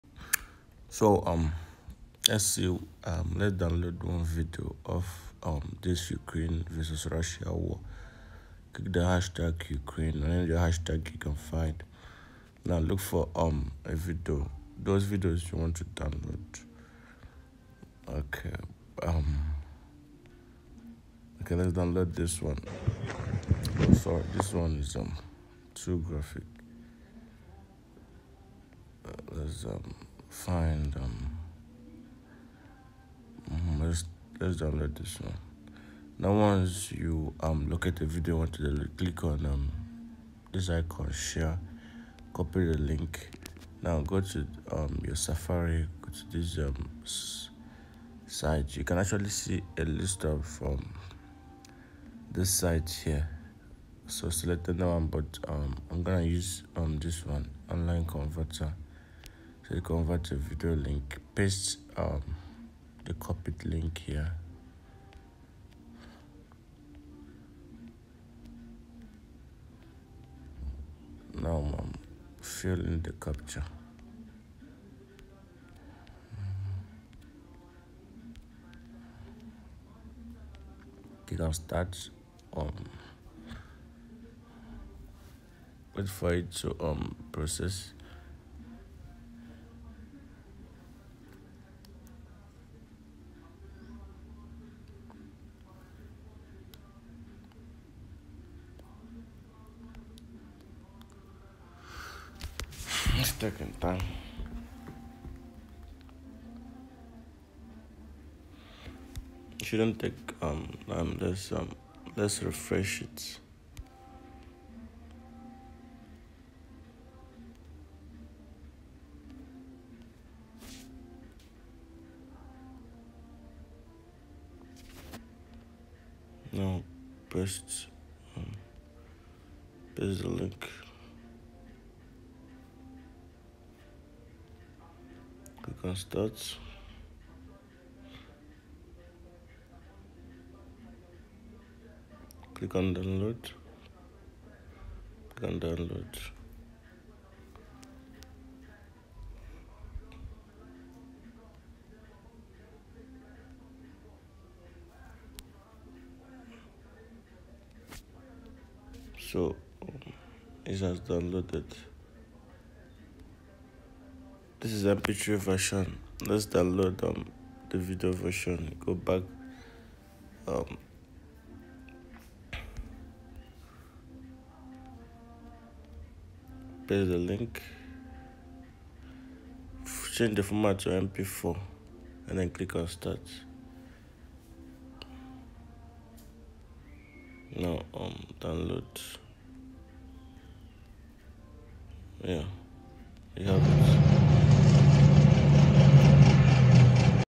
How to Save Twitter videos to IPhone’s camera roll
Easily download and save any Twitter video to your iPhone.